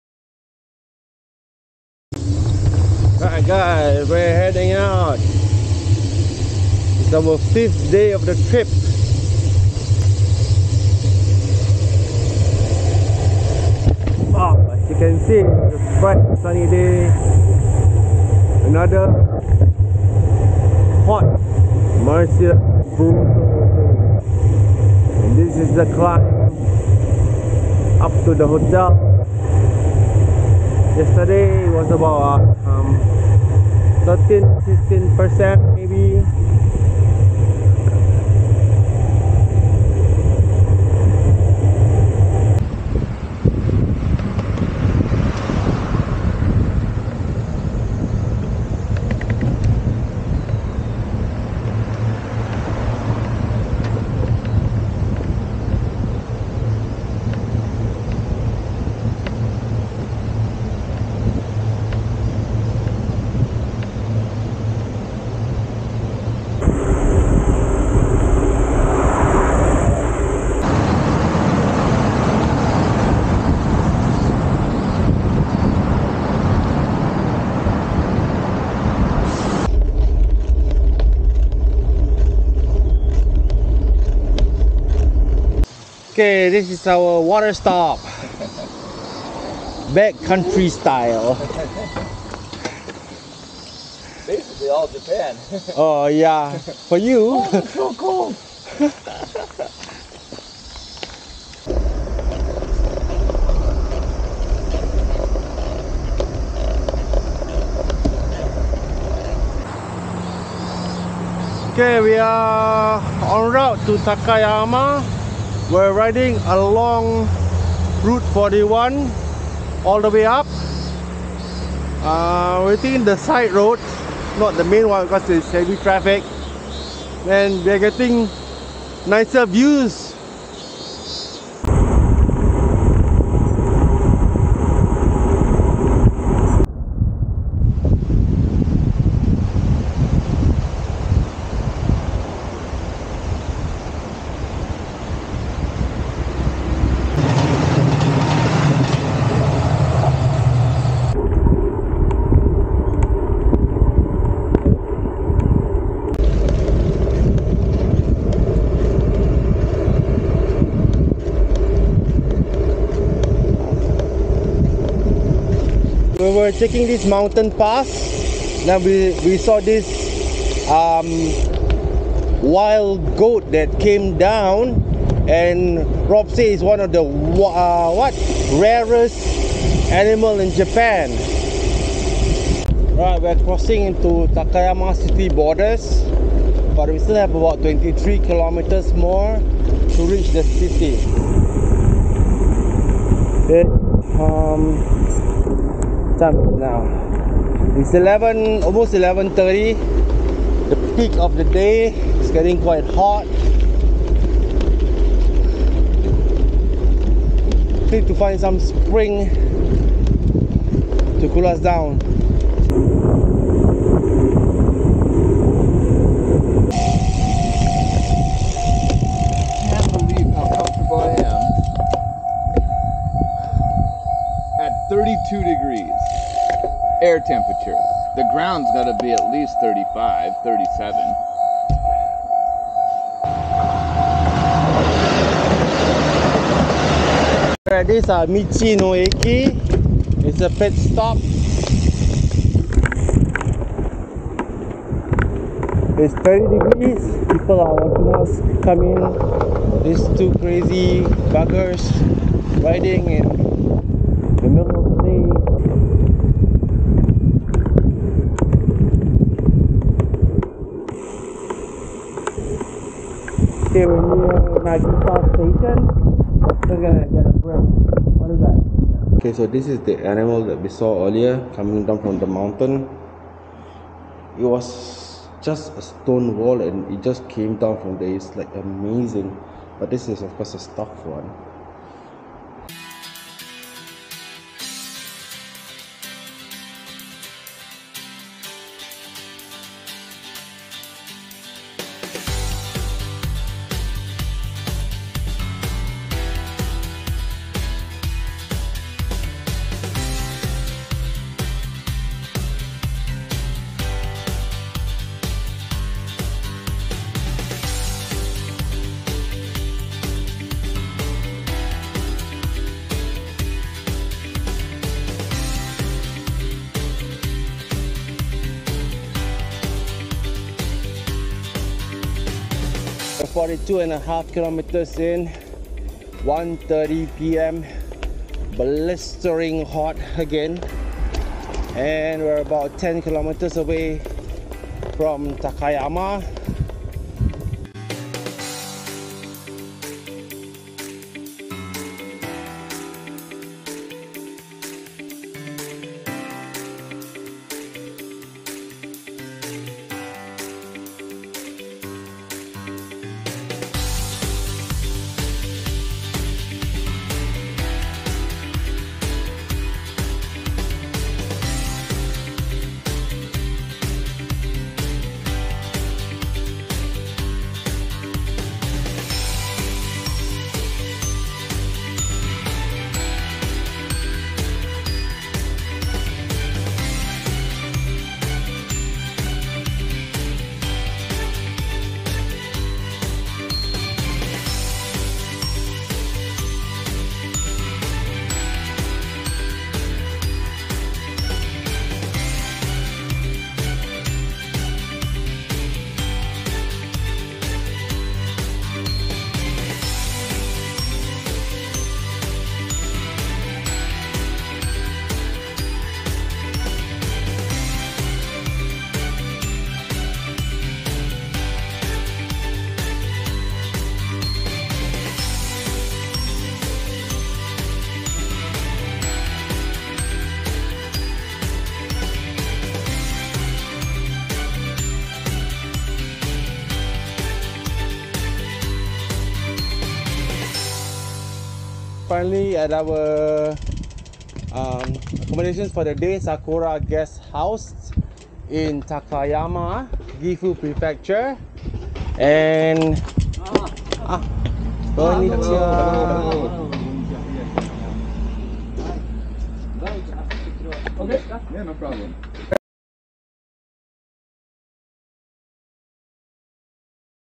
Day 5/6: Gero to Takayama, Gifu Prefecture. Another day of recovery ride.@robertcolqui1692
Left the hotel after a hearty breakfast. We did not have to rush as it was a short 54km recovery ride to Takayama. We rode along the river on National Road 41. We took the small roads whenever possible as there were fast moving traffic on the National Road. Going on the side road also means there is a higher change of natural spring water coming off the mountains to cool ourselves down.

There is a tunnel section before Nagisa where bicycles are not allowed through so we had to take a detour around it. We were in for a surprise when a Kamoshika or Japanese Serow, one of the rarest animals in Japan, came down from the mountains just 10 meters infront of us before jumping to the other side of the road. It happened so fast that I
didn't even managed to take a video of it.

It was getting really hot around mid-day so we stopped at Nagisa Eki no Michi or Nagisa stop to cool down for a while. We had a lot of stares as we came in. They were probably thinking these 2 guys are crazy to be cycling in this weather. There was an air-conditioned room with a stuff Kamoshika.

We continued after a short break and dunking our jackets in water, which kept us cool as cucumber, until we reach our accommodations for the night, Sakura Guest House, at about 3pm.

Checked in, showered, did laundry, went out to buy groceries, cooked our dinner and chat till I went to bed at about 10pm.

Sorry for not uploading for so long. Was away for summer vacation.